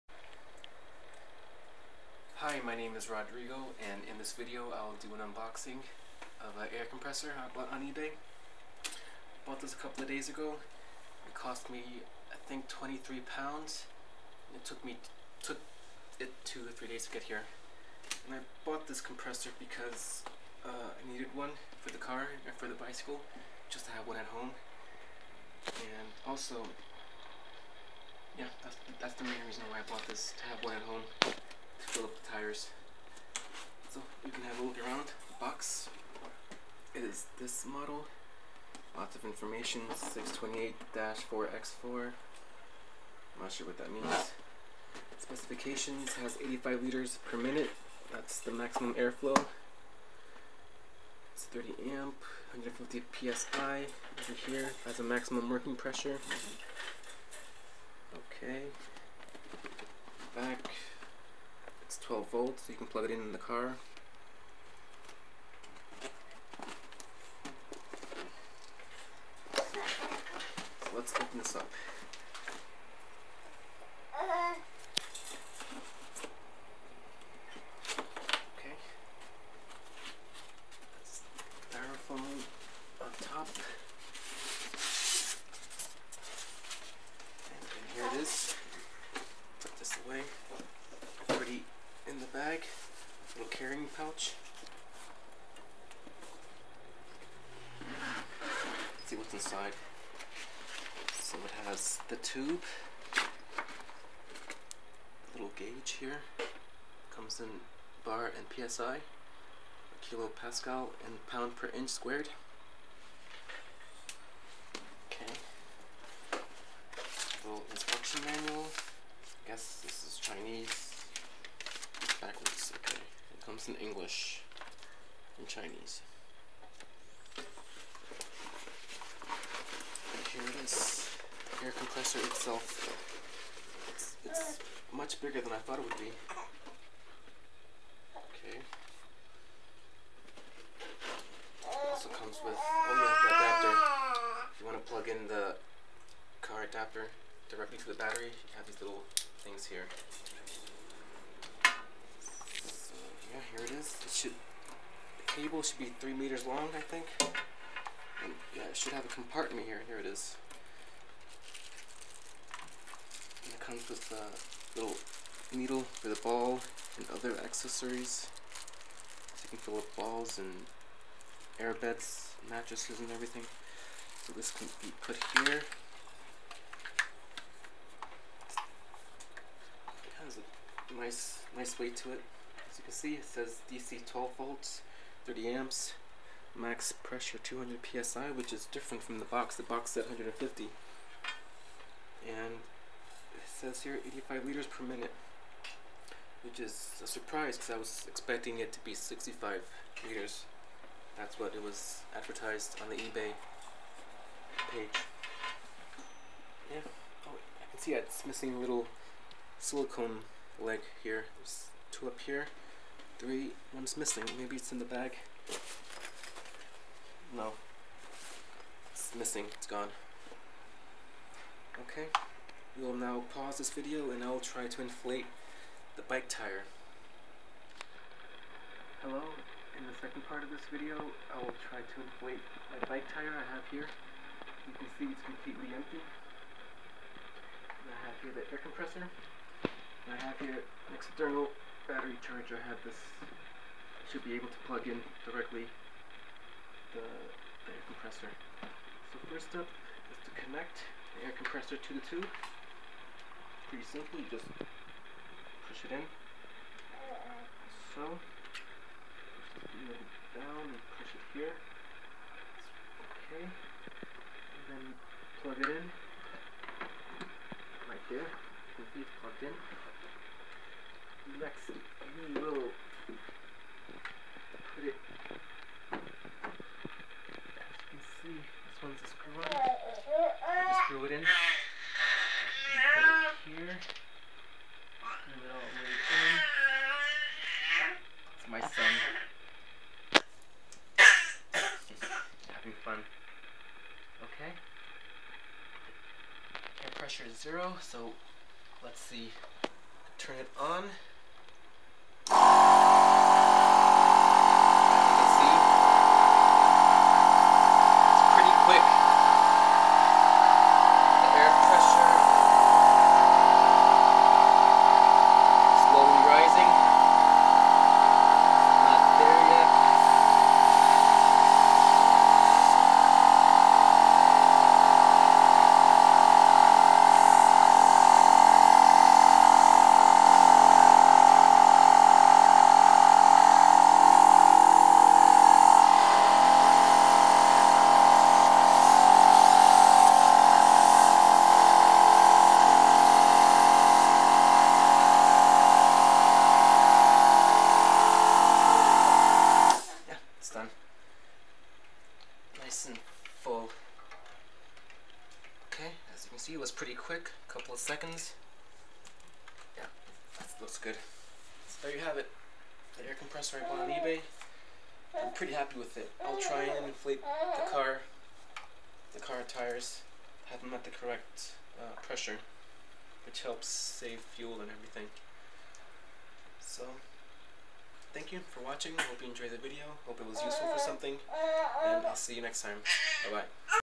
Unboxing 2 Cylinder Air Compressor 200 PSI 12V DC ebay
Hi, I do the unboxing and small trial of an air compressor I bought on eBay. I also give my initial impressions.

Model AC-628A